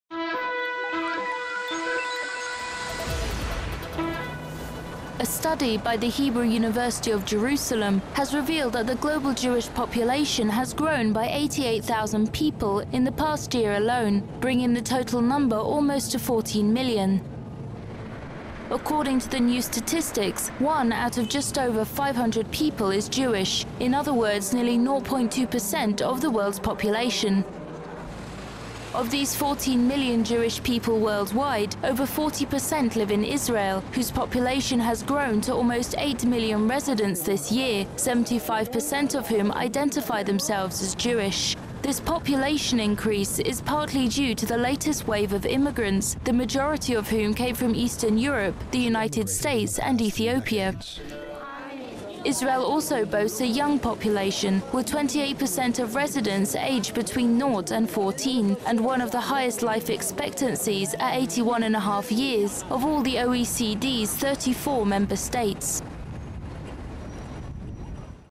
Hebrew University Jewish population stats
A study by the Hebrew University of Jerusalem has revealed that the global Jewish population has grown by 88,000 people in the past year alone, bringing the total number almost to 14 million.

According to the new statistics, 1 out of just over 500 people is Jewish, in other words nearly 0.2% of the world's population.

Of these 14 million Jewish people worldwide, over 40% live in Israel, whose population has grown to almost 8 million residents this year, 75% of whom identify themselves as Jewish. This population increase is partly due to the latest wave of immigrants, the majority of whom came from Eastern Europe, the US and Ethiopia.

Israel also boasts a young population, with 28% of residents aged between 0-14 and one of the highest life expectancies at 81.5 years of all the OECD's 34 member states.